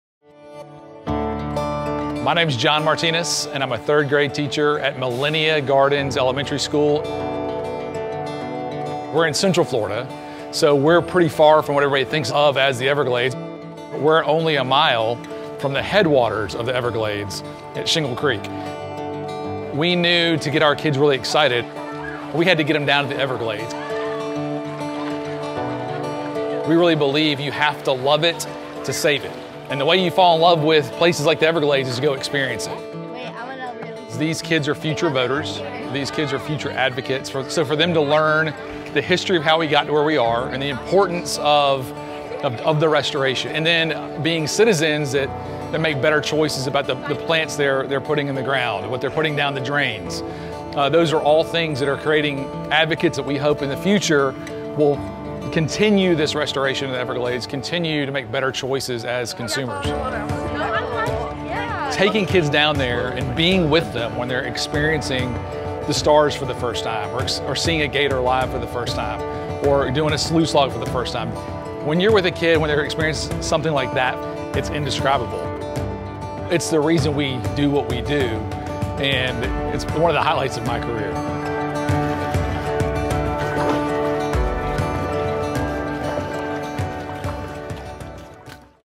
Orlando Teacher on Taking Students Out in Nature
We ❤️ our Everglades Teachers and we thank all Florida teachers who in and out of the classroom teach students about this amazing ecosystem. 🐊

"You have to love it, to save it, and the way you fall in love with places like the Everglades is to go experience it," says Orlando Millennia Gardens Elementary teacher, John Martinez.

You too can teach and learn about the Everglades! Visit evergladesliteracy.org to learn more about our Education program and how you school can get involved!